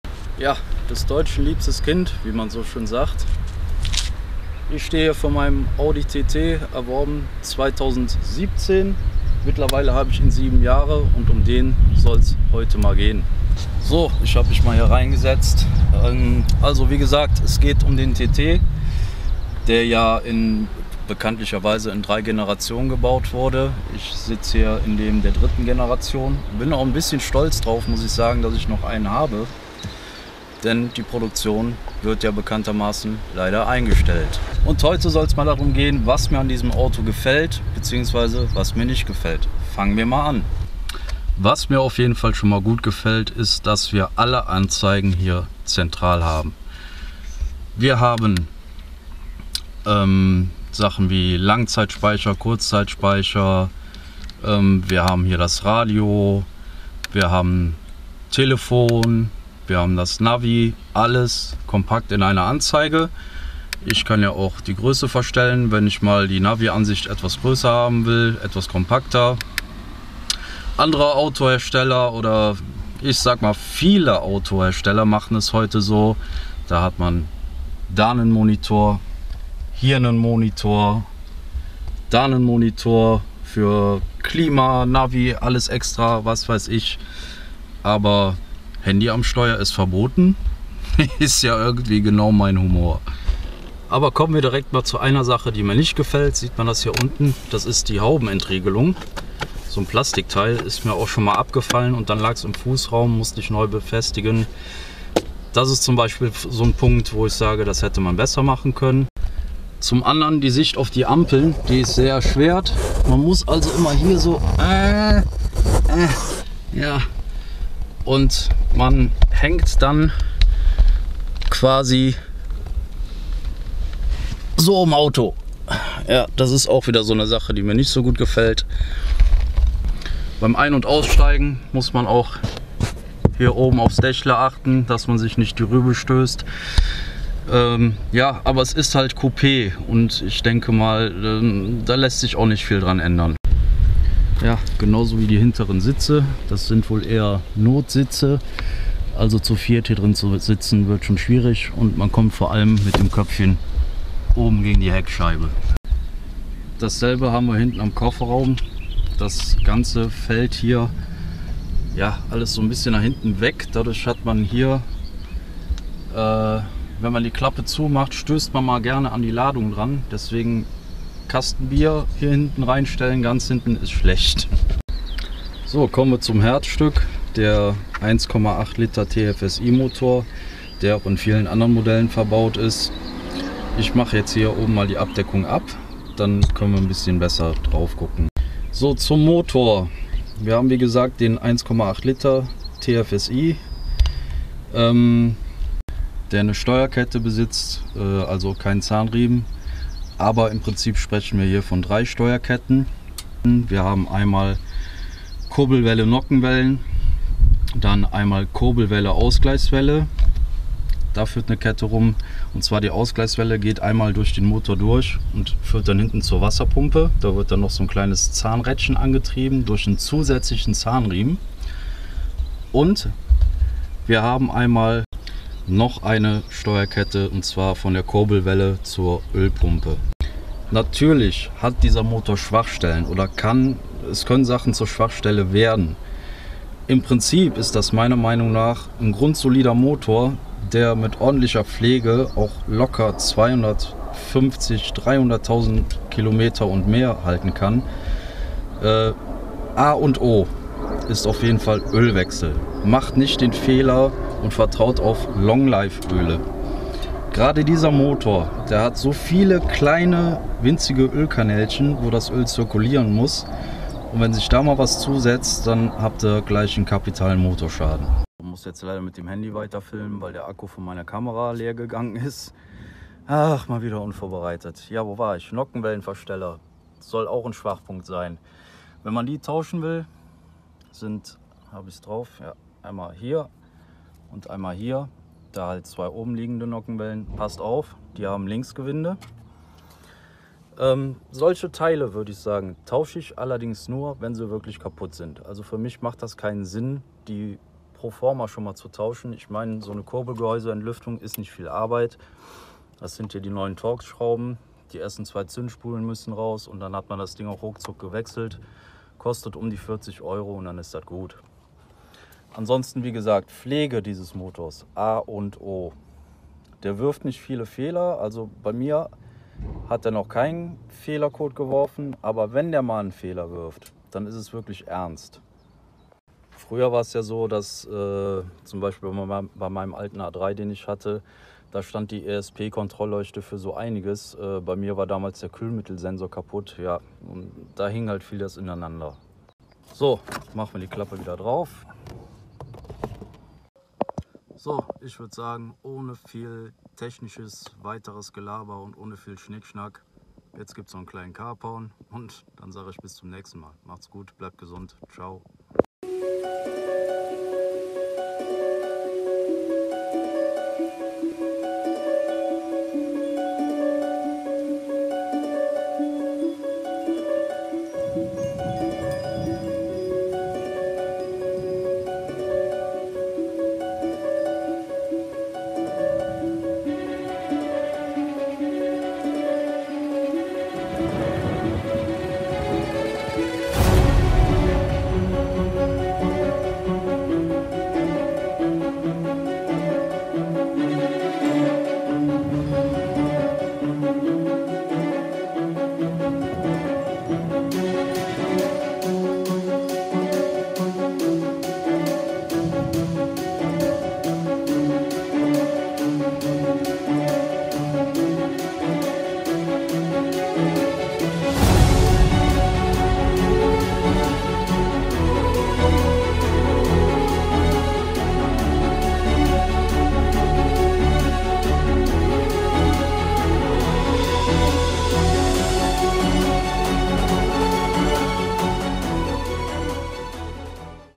Was mir an meinem TT gefällt / nicht gefällt
Kritik,Ergänzungen und was ich vergessen habe könnt ihr mir natürlich gern in die Kommentare schreiben.Apropos vergessen und um noch etwas vorweg zu nehmen : Der Motor hat die interne Bezeichnung EA 888.Und ja,die Aufnahmen sind nicht perfekt.Irgendwas ist ja immer ;-)